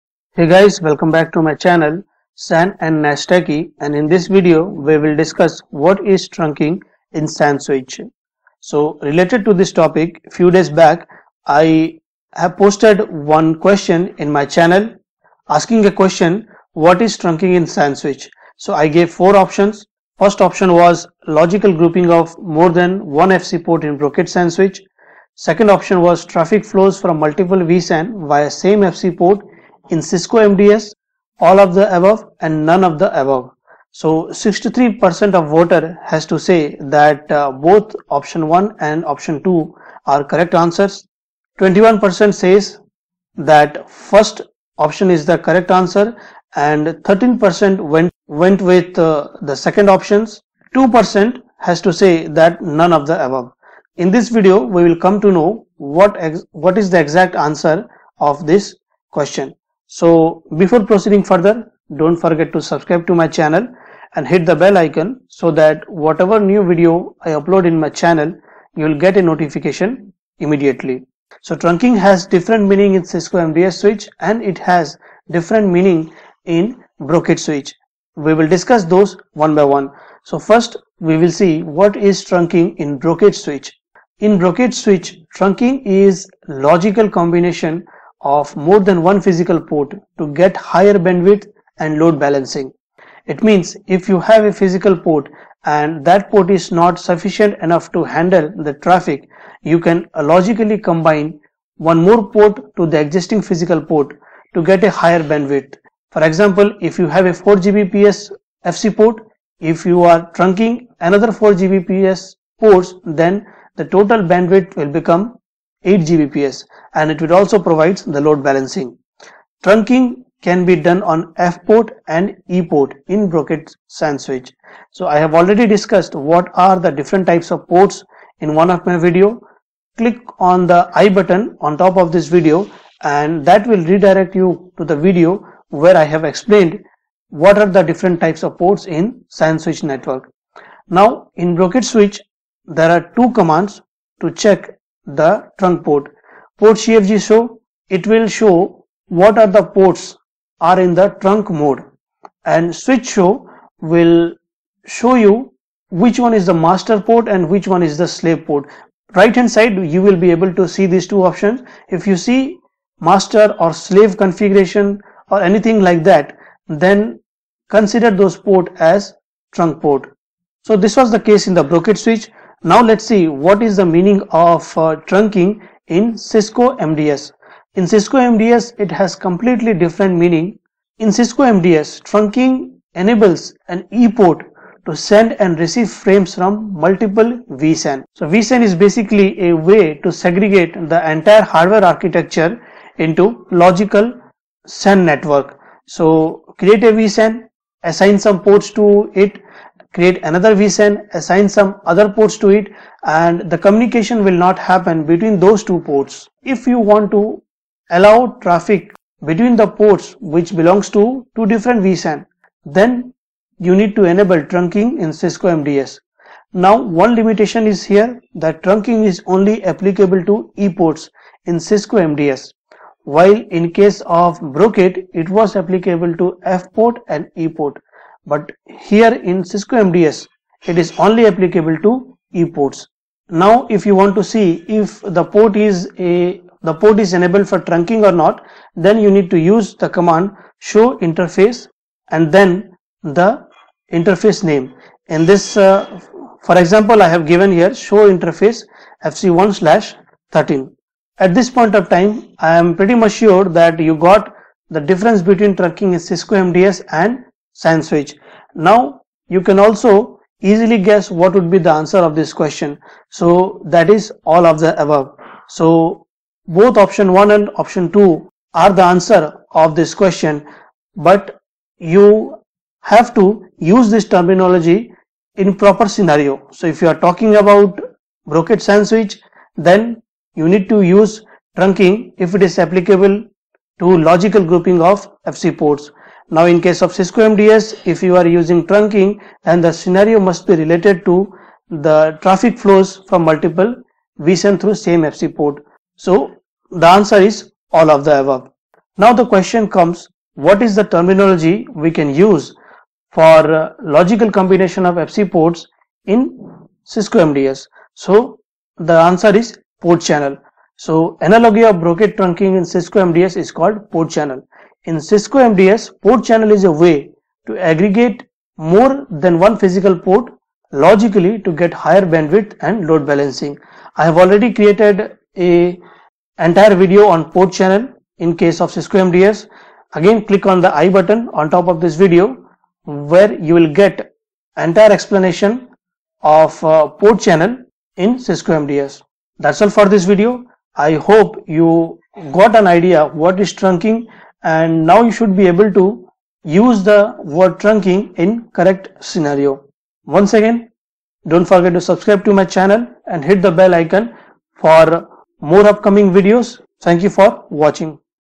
What Is Trunking In Cisco MDS and Brocade SAN Switch
What is trunking in Brocade SAN Switch?
In brocade, trunking is a logical combination of more than one physical port to get higher bandwidth and load balancing. We can enable Trunking in both  F and E port.

What is trunking in the Cisco SAN Switch?
In Cisco, MDS Trunking enables an E Port to send and receive frames from multiple VSAN. Trunking is only applicable to E ports in Cisco MDS Switch.